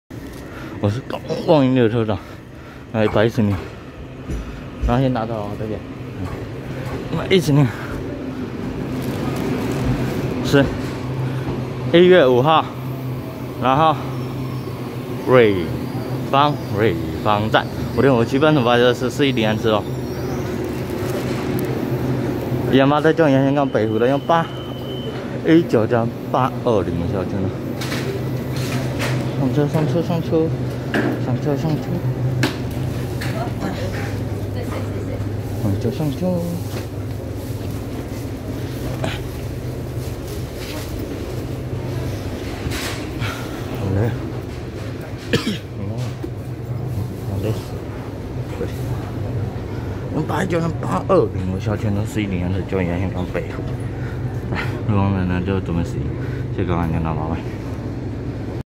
4103次宜蘭發車經由宜蘭線開往北湖的EMU819+EMU820微笑號區間車
110/11/5瑞芳站5:57分拍攝4103次宜蘭發車經由宜蘭線開往北湖的EMU819+EMU820微笑號區間車